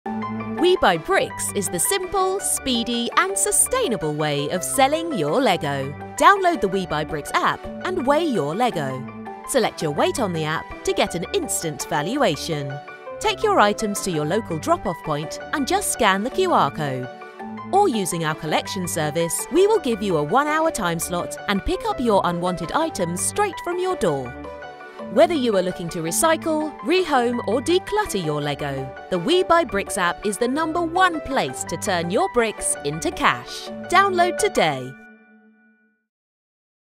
WeBuyBricks | Promo Explainer - How it works
From concepts to filming and post production, we produced this concise, informative promo video explaining how the WeBuyBricks process works for the customer.